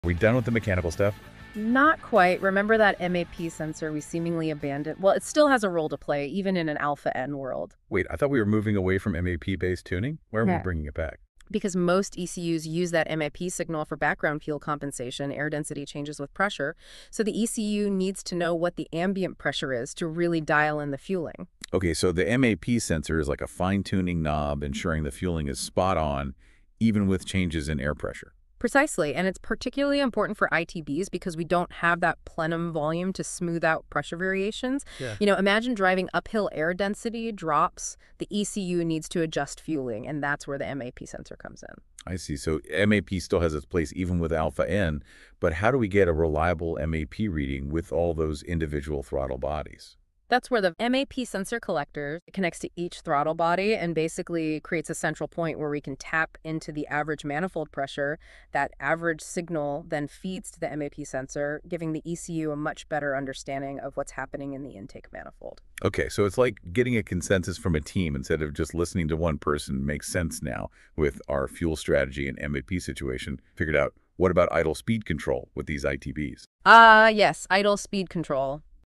MAP Sensor's Role in ITB Setups Explained!
Wait, I thought ITBs use Alpha-N tuning? Why do we still need a MAP sensor? 🤔

Even in Alpha-N setups, your MAP sensor plays a CRUCIAL role:
- Provides essential air density data for fuel compensation
- Helps your ECU adjust for elevation changes
- Ensures proper fueling as ambient pressure varies

The trick? A MAP collector connecting to EACH throttle body to get an average pressure reading! 📈

Without this, your carefully tuned ITB setup could run lean at higher elevations or rich at lower ones.

Small detail, MASSIVE impact on drivability and reliability! 💯